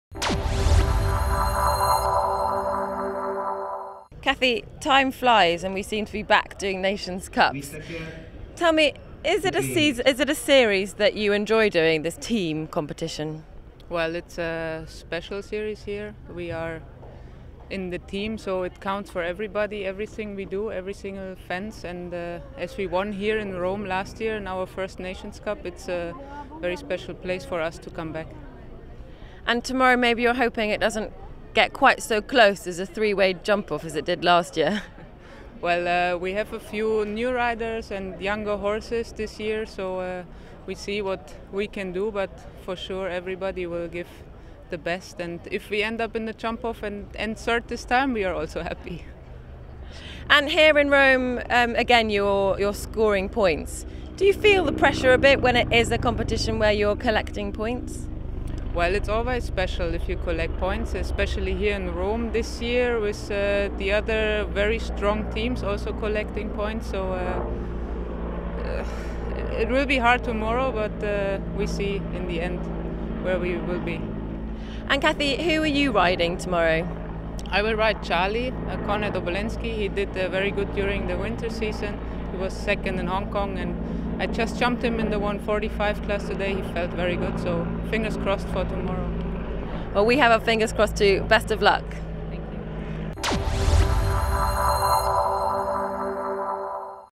Furusiyya FEI Nations Cup™ Jumping 2014 - Rome - Kathy Offel
Ukraine will be hoping for a repeat performance from last year as Kathy Offel and her team try to earn points.